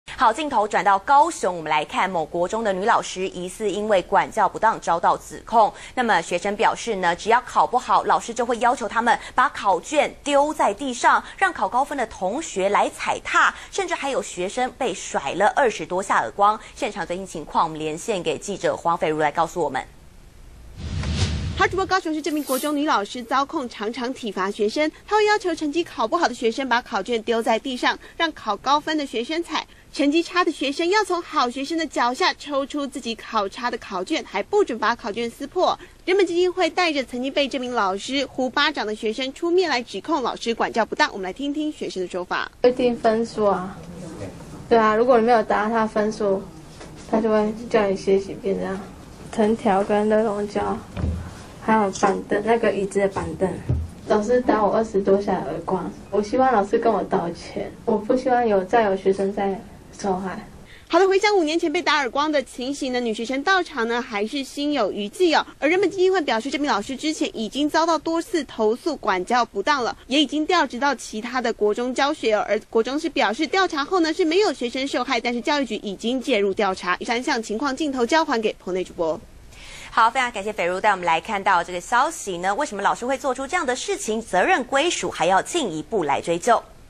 成績差考卷丟地踩 師辱生：不准撕破
中天新聞》鳳山區某國中莊姓女教師被控5年前在大樹國中任教時體罰學生，學校與教育局均未處理；2年多前，介聘到鳳山區某國中，最近又遭家長投訴體罰，人本基金會11月30日陪當年被甩20多下耳光女學生出面控訴，要求教育局查明處置，還學生一個公道。